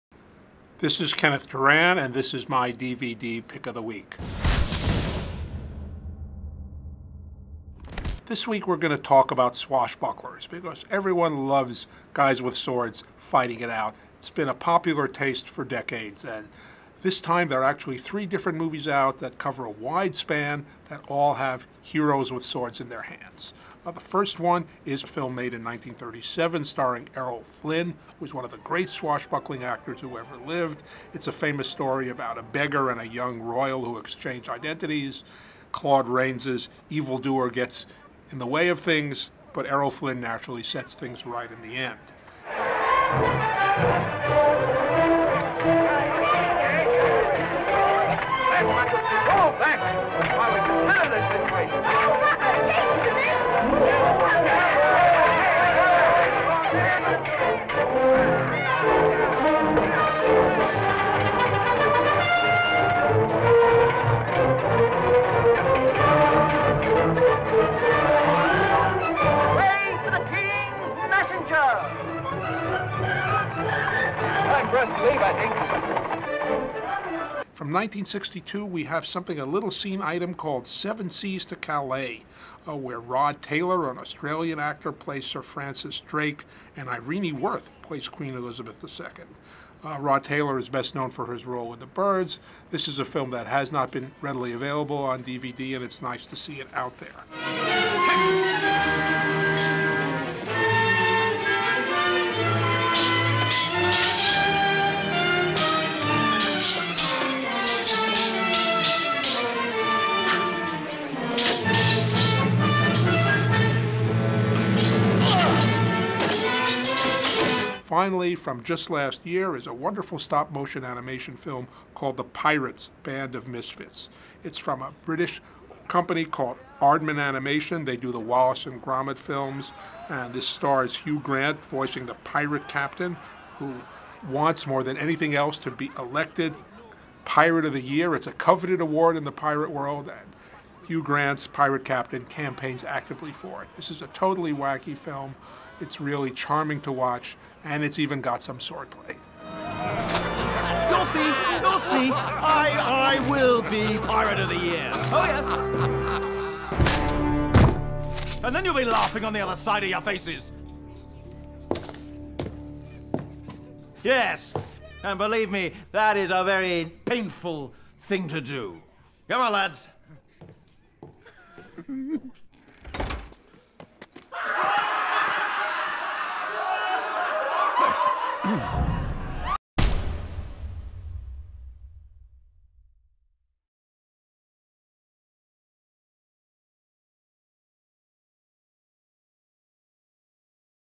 Kenneth Turan's DVD pick of the week - Swashbucklers
Kenneth Turan picks 3 different movies with a swashbuckling theme.